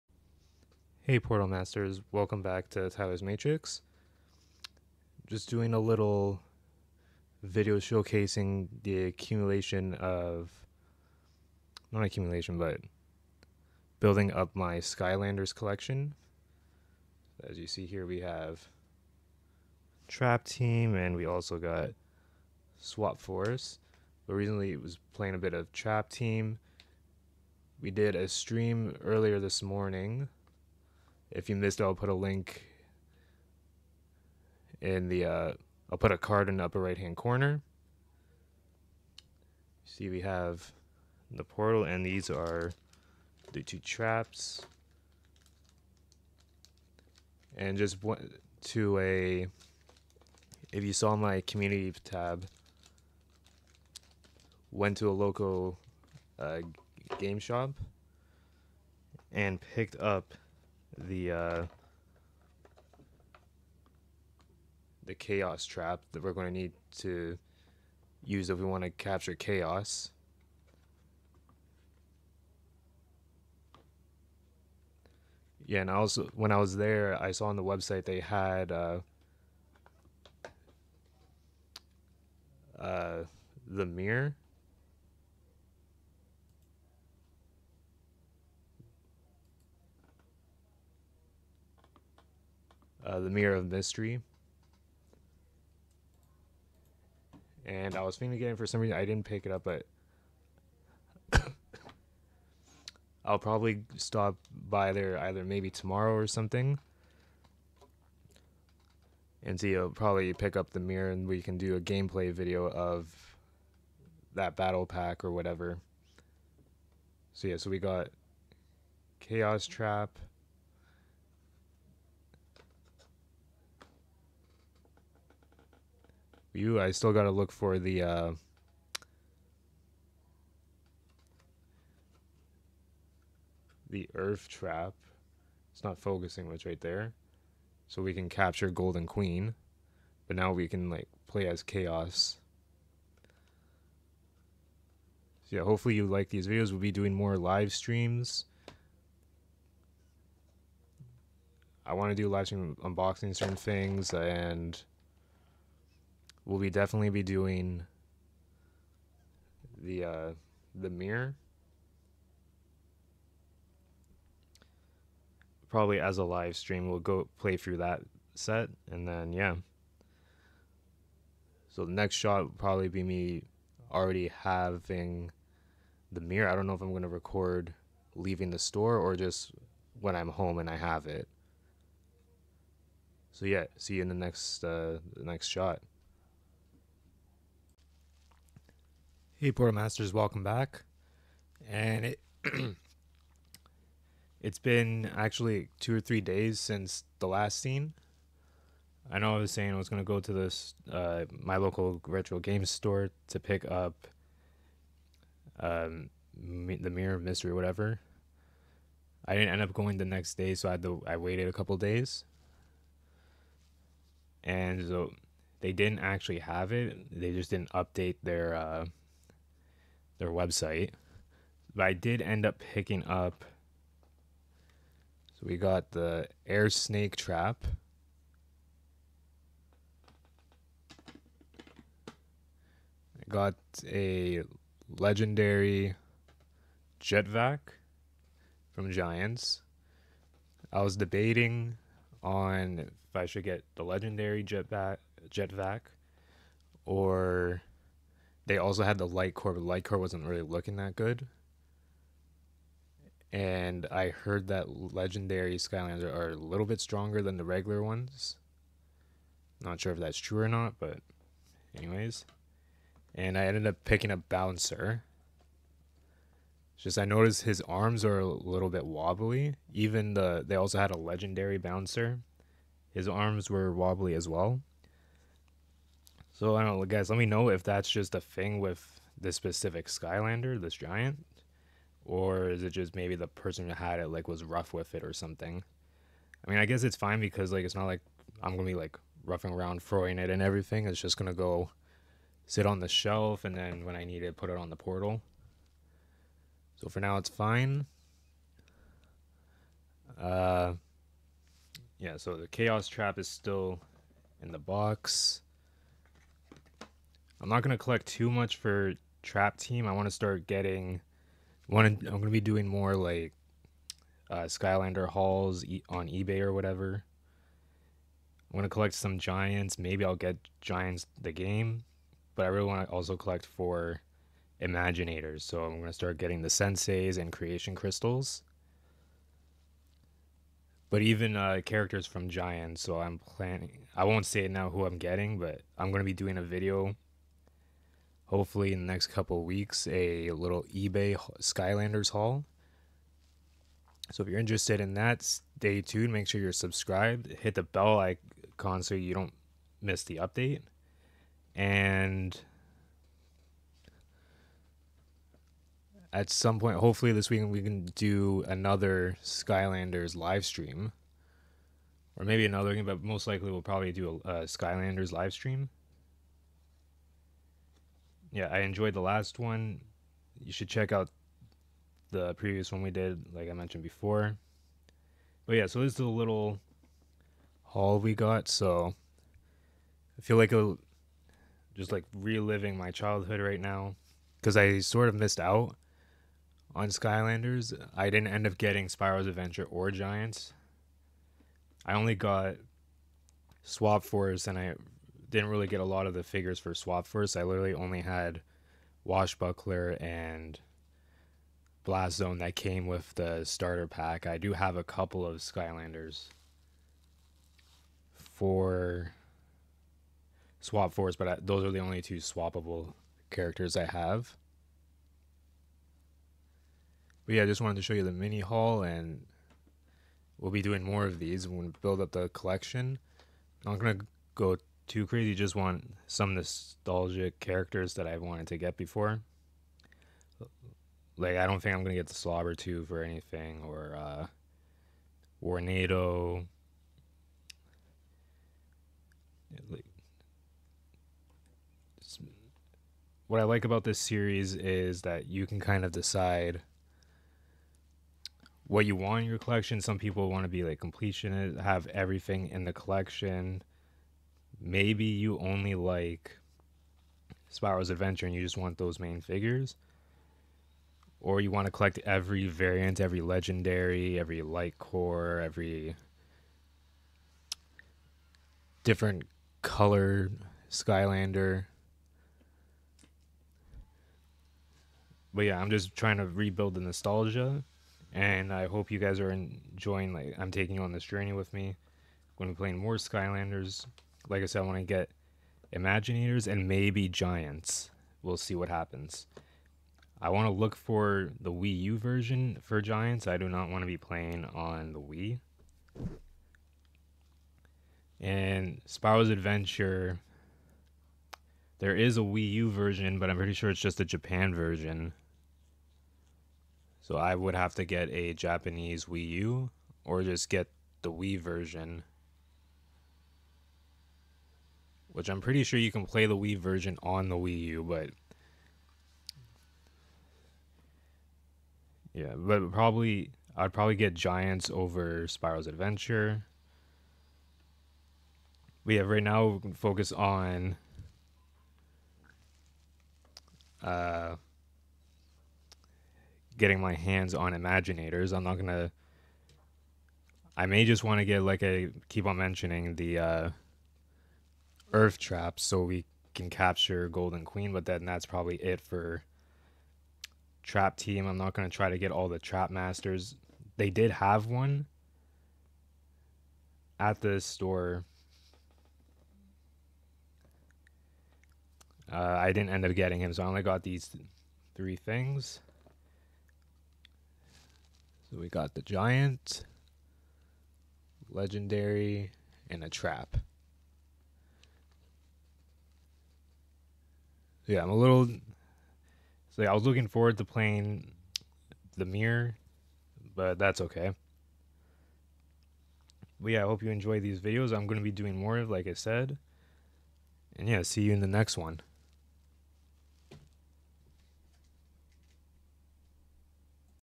ADDING TO THE SKYLANDERS COLLECTION
Hello fellow Portal Masters!  I'm slowly building up my Skylanders collection. I made this video so I can take you guys on this journey with me! Rebuilding the nostalgia and my childhood, I've picked up some classic Skylander figures.